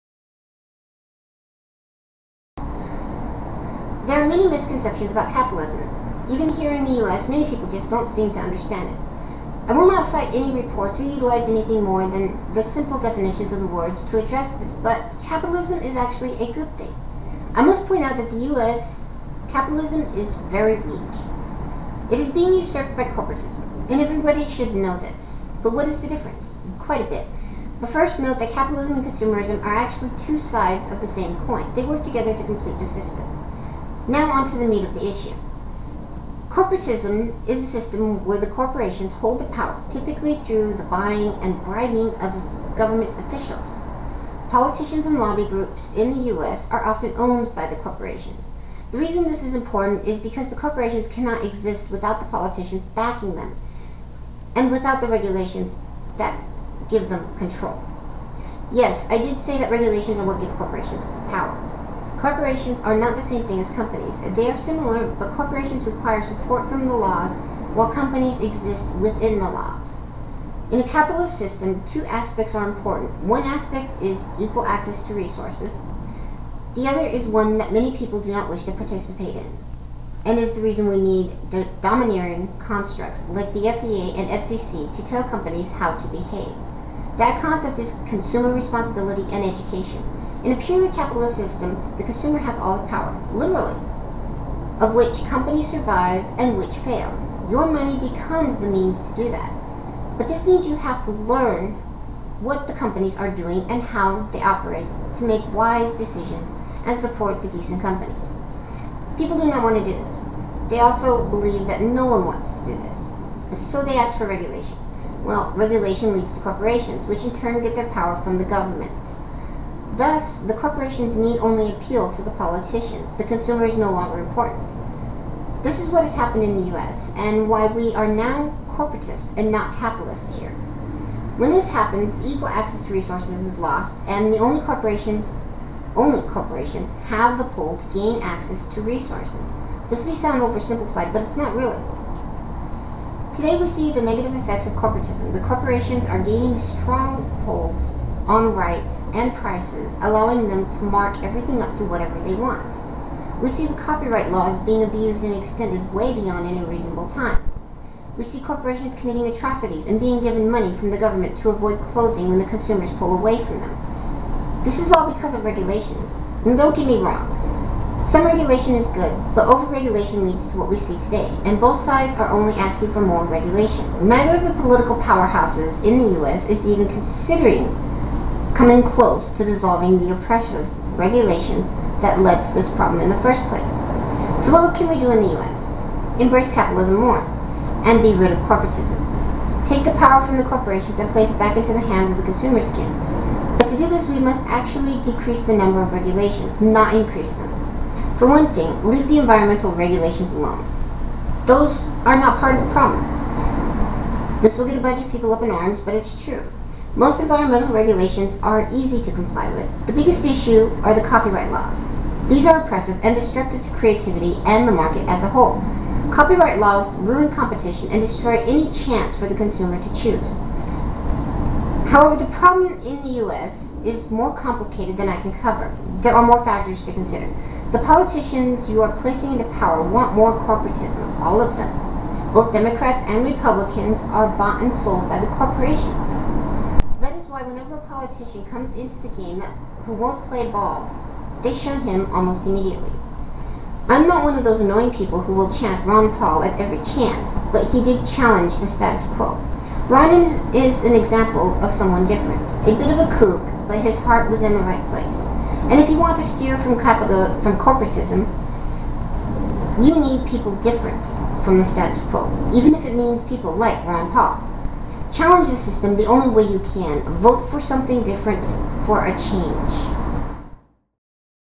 Capitalism, a Scientific Approach
Taking a bit of a break from advocate because this idea just jelled in my brain to something resembling a coherent thought. If you want to debate politics, send a video response, I love to debate politics without the numbers.

Oh, and yeah, I stumbled a few time, it was like one long tongue twister.